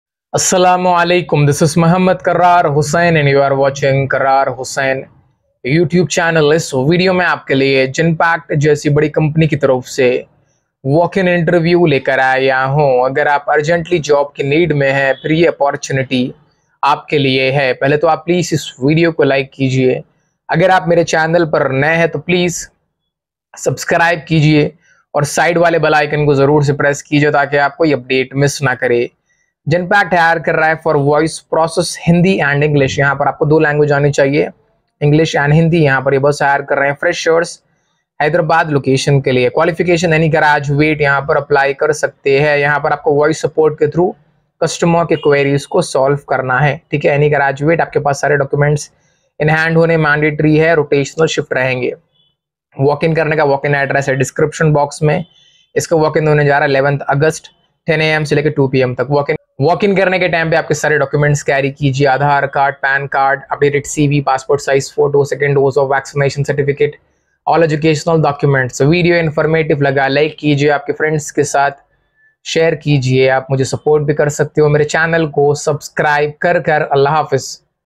Genpact Off Campus Placement 2023 | Onspot Offer Letter | Genpact Hiring Freshers | Genpact Jobs
🔴GENPACT WALKIN ADDRESS:
Time and Venue
11th August, 10.00 AM - 2.00 PM
Address: F9P5+3FV, Hafeezpet Rd, Vinayaka Nagar, Hafeezpet, Hyderabad, Telangana 500049
ON TOP OF CV WRITE - Nawaz khan ()

Images & Jobs are taken from the following :
2.GENPACT
3.Naukri.com